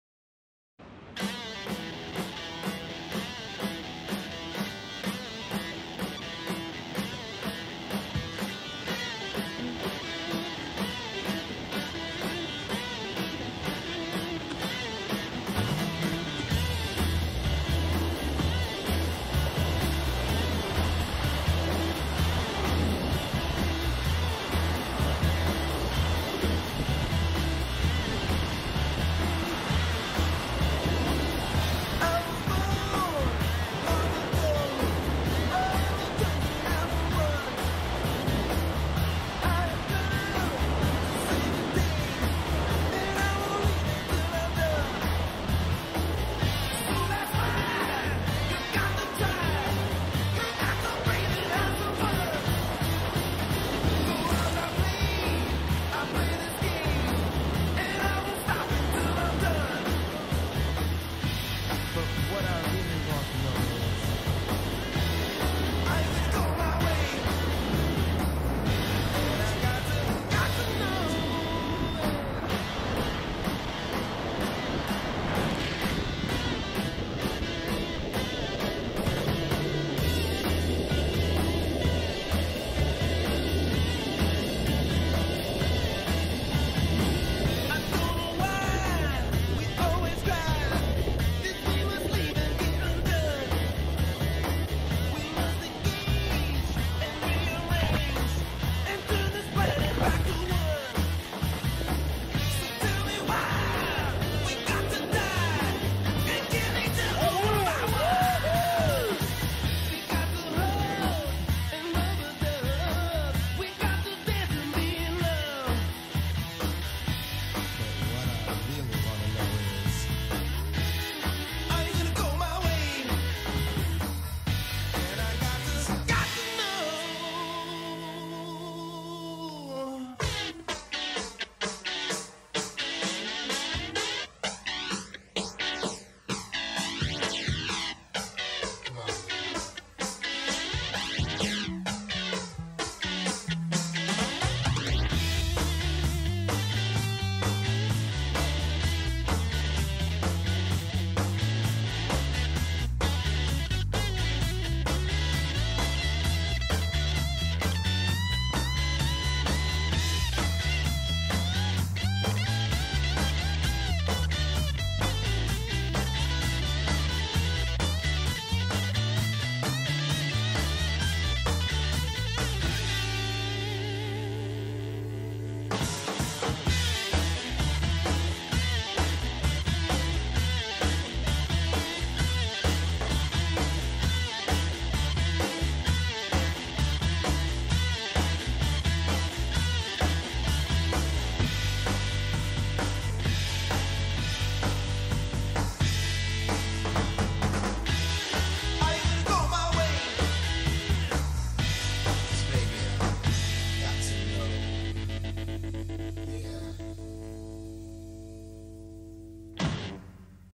More Grand Canyon Rapids ~ 2021
Hermit and Dubendorf Rapids 1/21. Plus a tasty tune by Lenny Kravitz.